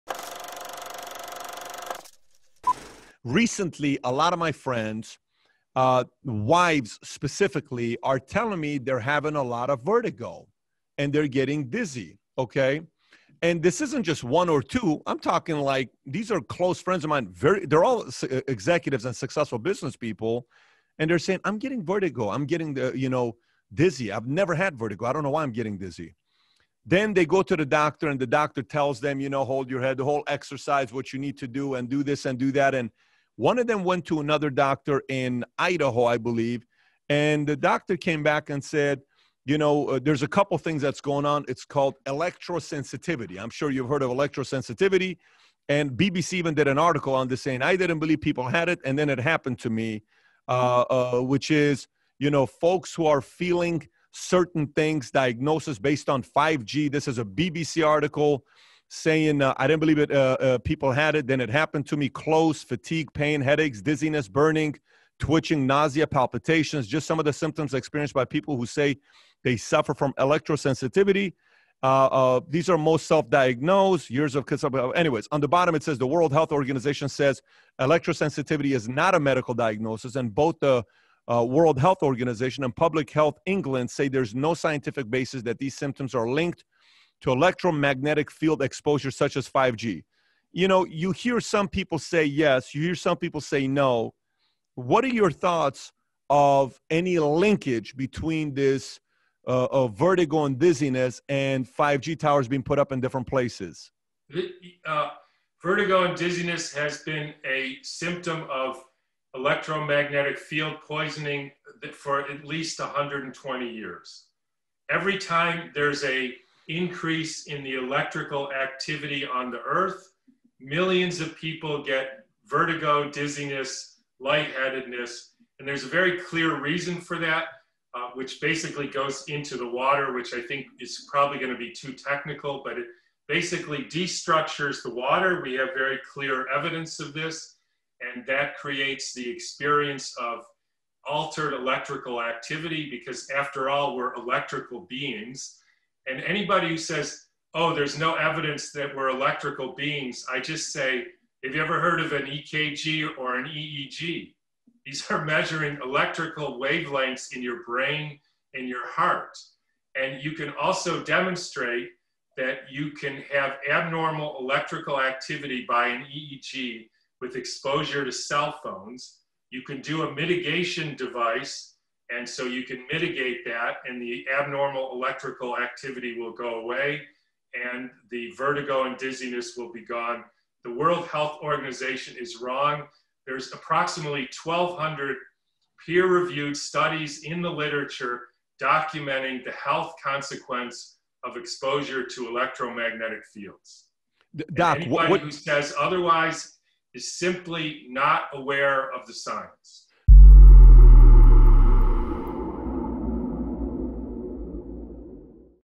Does 5G Cause Vertigo?
Patrick Bet-David sits down with MD Thomas Cowan to talk about health and medicine. In this clip they talk about the connection between vertigo and 5G.

About the guest: Thomas Cowan, MD, has studied and written about many subjects in medicine including nutrition, homeopathy, anthroposophical medicine, and herbal medicine. He is author of Human Heart, Cosmic Heart, the principal author of The Fourfold Path to Healing and co-author (with Sally Fallon) of The Nourishing Traditions Book of Baby and Child Care.

Dr. Cowan has served as vice president of the Physicians’ Association for Anthroposophic Medicine and is a founding board member of the Weston A. Price Foundation. He also writes the “Ask the Doctor” column in Wise Traditions in Food, Farming, and the Healing Arts (the Weston A. Price Foundation’s quarterly magazine) and has lectured throughout the United States and Canada.

About Patrick Bet-David: CEO, author and Founder of Valuetainment Media. Patrick has interviewed athletes, notorious individuals, politicians, authors and entrepreneurs from all walks of life.